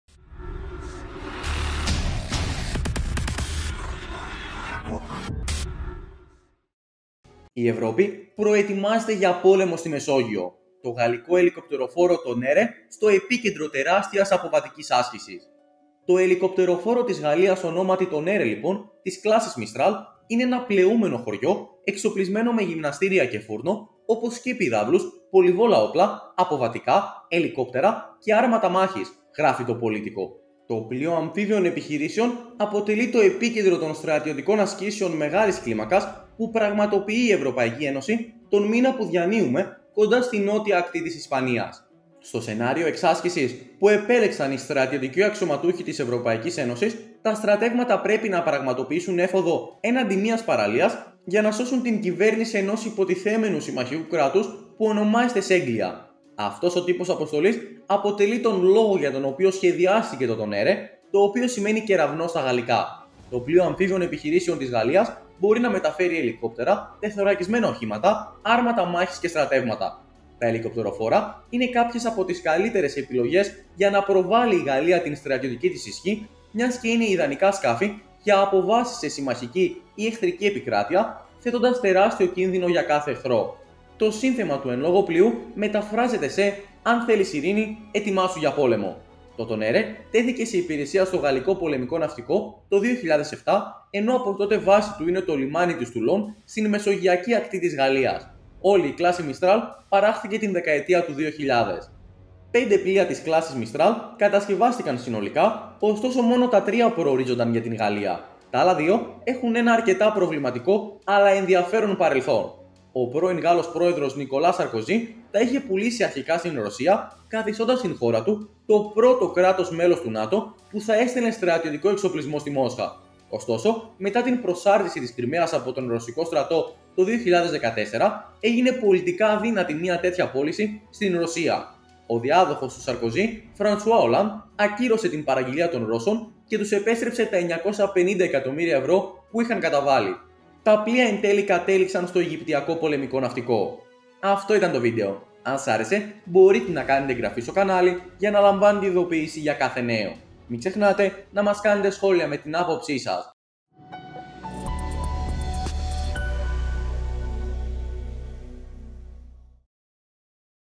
Η Ευρώπη ετοιμάζεται για σφοδρό πόλεμο στην Μεσόγειο! Τεράστιες αποβατικές ασκήσεις στην Ισπανία
Η Ευρώπη προετοιμάζεται για πόλεμο στη Μεσόγειο!

Το γαλλικό ελικοπτεροφόρο Tonnerre στο επίκεντρο τεράστιας αποβατικής άσκησης

Στόχος της οποίας είναι η θεωρητική διάσωση κυβέρνησης συμμαχικού κράτους της δυτικής συμμαχίας!

Κεφάλαια:
0:00 - Εισαγωγή
01:41 - Τα Ήθελαν οι Ρώσοι Αλλά Ποτέ δεν τα Πήραν